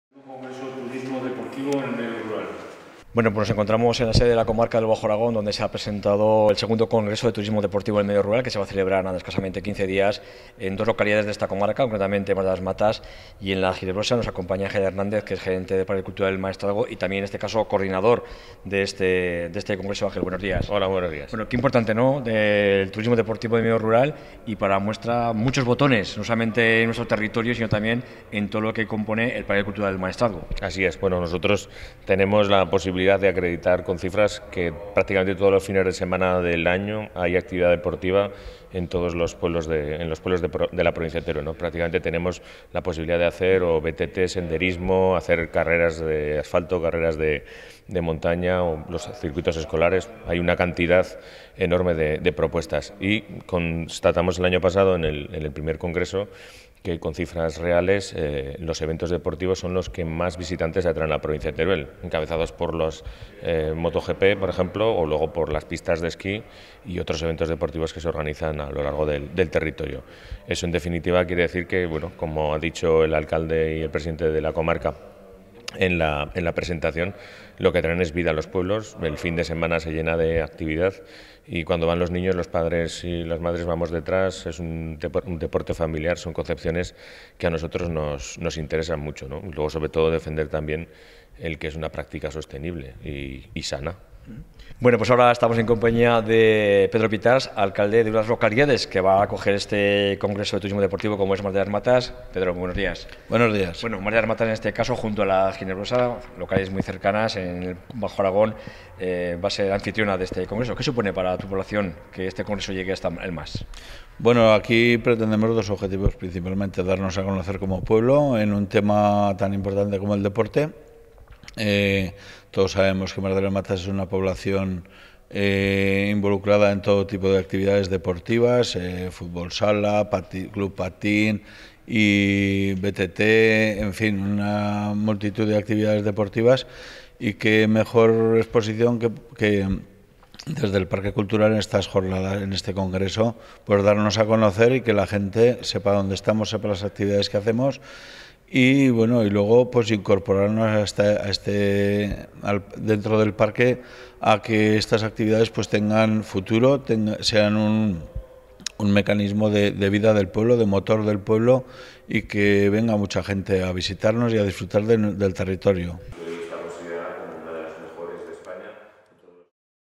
Próximo Congreso Turismo Deportivo
Durante los días 18, 19 y 20 de octubre las localidades de Mas de las Matas y La Ginebrosa acogerán el II Congreso de Turismo Deportivo en el Medio Rural que organiza el Parque Cultural del Maestrazgo con la colaboración de la Comarca del Bajo Aragón.